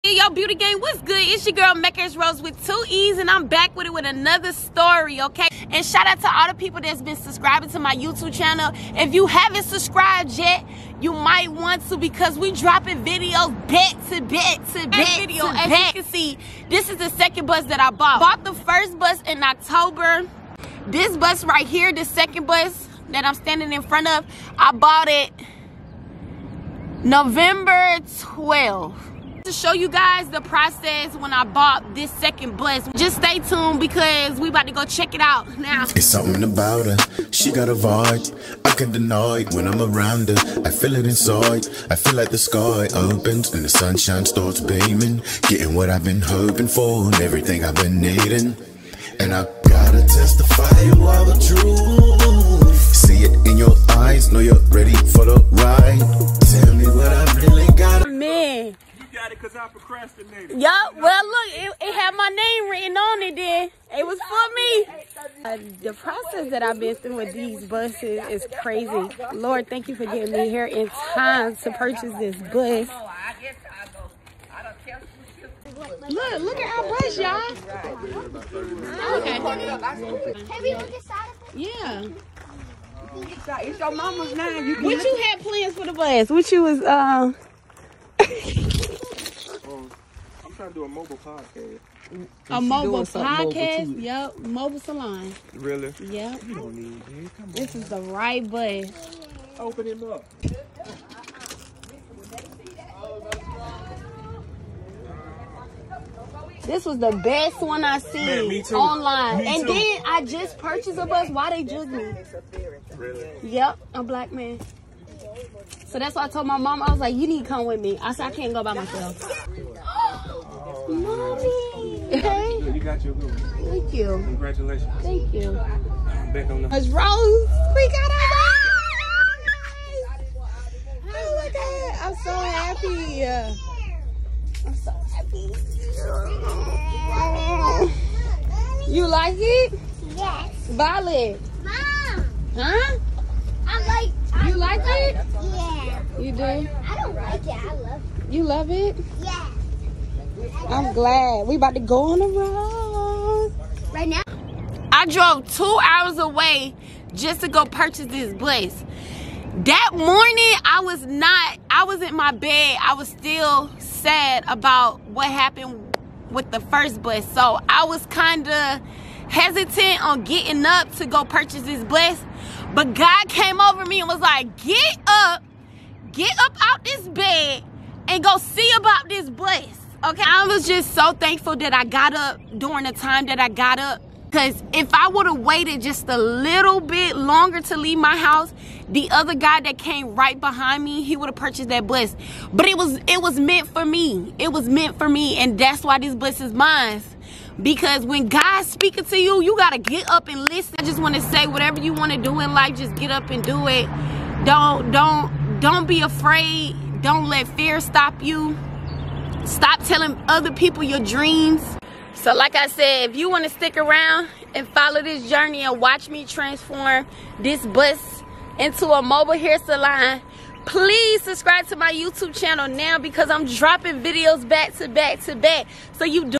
Purchasing my second school bus!!
Check out this vlog of me purchasing my second school bus!! Thank you God for allowing me to get up in time to purchase my baby.!! My kids and I was so excited !!

Beauty Gang our next video will BE DROPPING SOON!!